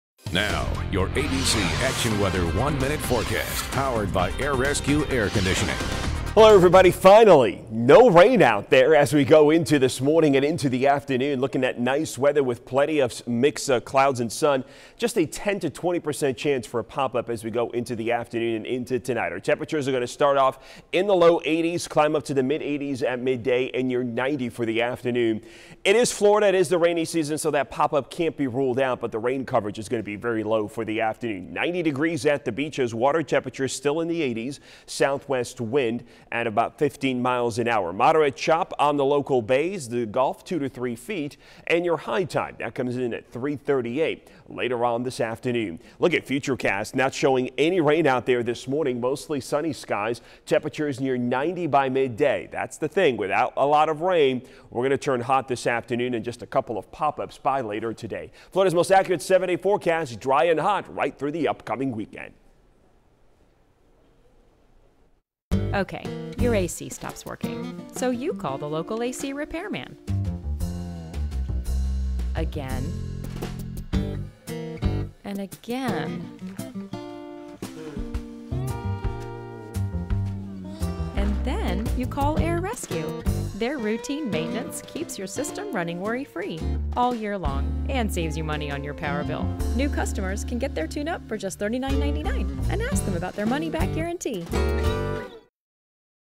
Florida's Most Accurate Forecast with Greg Dee on Thursday, June 20, 2019
Mostly sunny with only a slight chance of a PM pop up. Low 90s.